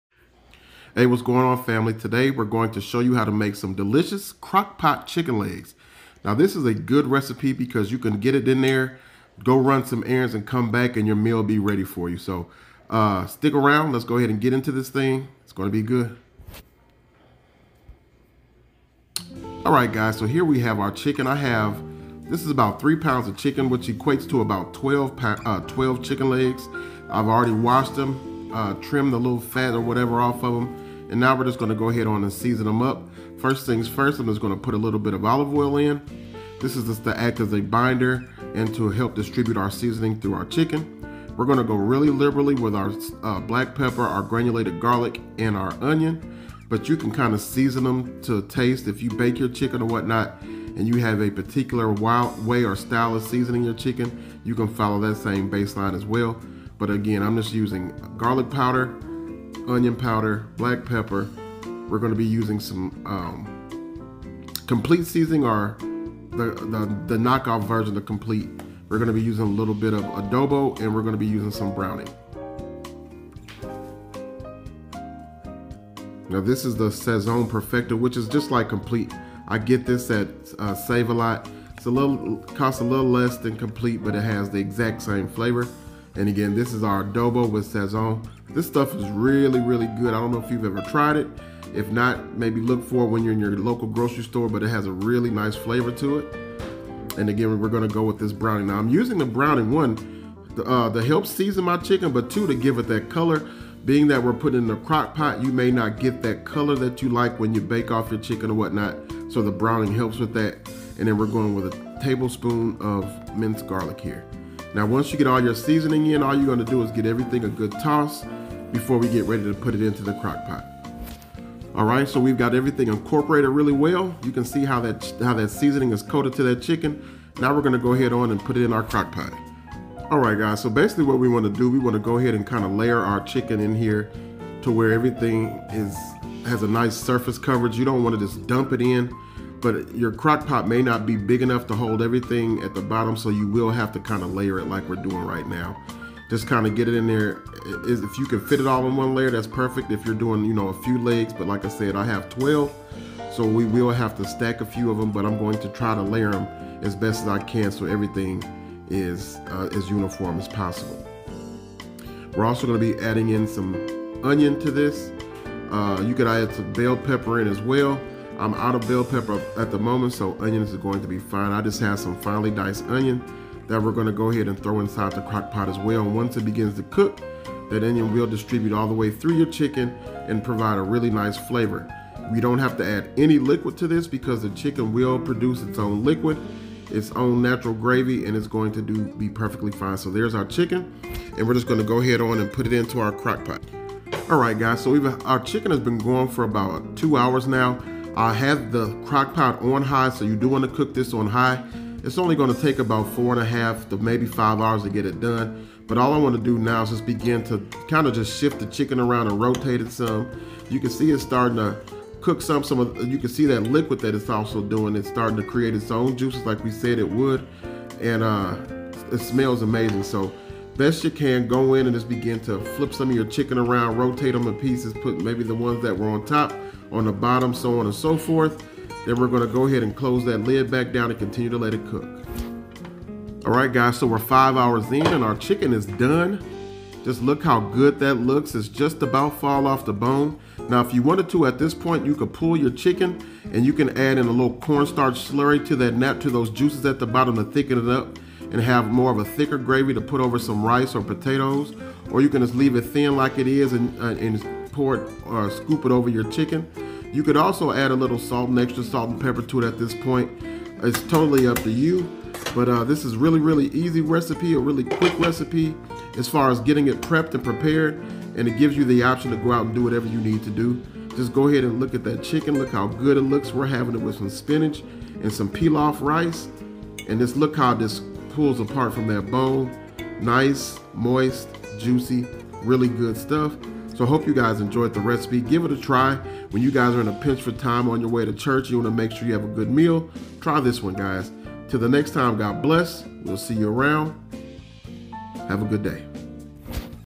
You'll definitely want to try this. Crock Pot chicken Legs. So easy, so good.
Do you wanna make sure you have a good hot meal? Waiting for you when you get back from running errands or working? I've got one for you.These quick and easy yet delicious crock pot chicken legs will do the trick. I hope you guys give this one a try. GOD bless.